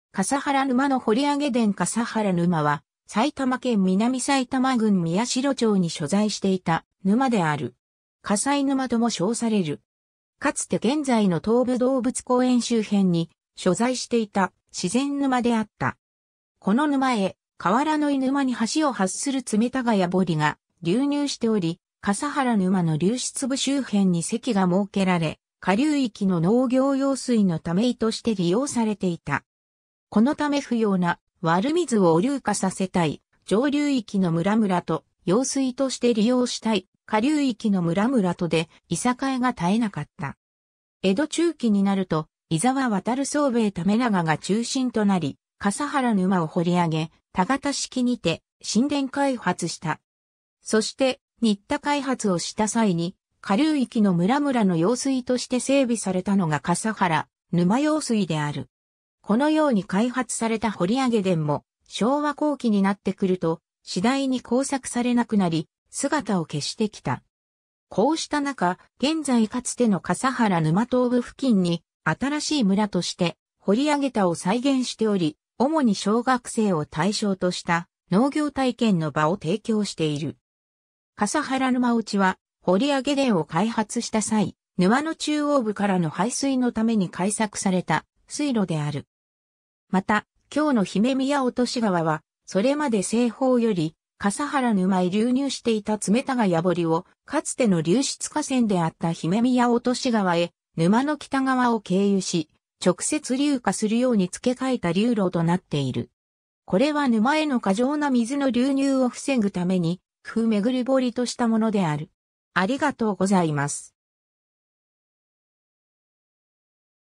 笠原沼
笠原沼の掘り上げ田（2011年12月） 笠原沼（かさはらぬま）は、埼玉県南埼玉郡宮代町に所在していた沼である。
笠井沼（かさいぬま）とも称される。
 かつて現在の東武動物公園周辺に所在していた自然沼であった。
この沼へ河原井沼に端を発する爪田ヶ谷堀が流入しており、笠原沼の流出部周辺に堰が設けられ下流域の農業用水の溜井として利用されていた。
このため不要な悪水を流下させたい上流域の村々と、用水として利用したい下流域の村々とでいさかいが絶えなかった。
江戸中期になると井沢弥惣兵衛為永が中心となり、笠原沼を掘り上げ田形式にて新田開発した。
そして、新田開発をした際に下流域の村々の用水として整備されたのが笠原沼用水（笠原沼代用水）である。
このように開発された掘り上げ田（ホッツケ）も昭和後期になってくると次第に耕作されなくなり、姿を消してきた。
こうした中、現在かつての笠原沼東部付近に「新しい村」として掘り上げ田を再現しており、主に小学生を対象とした農業体験の場を提供している。
 笠原沼落は掘り上げ田を開発した際、沼の中央部からの排水のために開削された水路である。
また、今日の姫宮落川は、それまで西方より笠原沼へ流入していた爪田ヶ谷堀をかつての流出河川であった姫宮落川へ沼の北側を経由し、直接流下するように付け替えた流路となっている。
これは沼への過剰な水の流入を防ぐために附廻堀（笠原付廻堀）としたものである。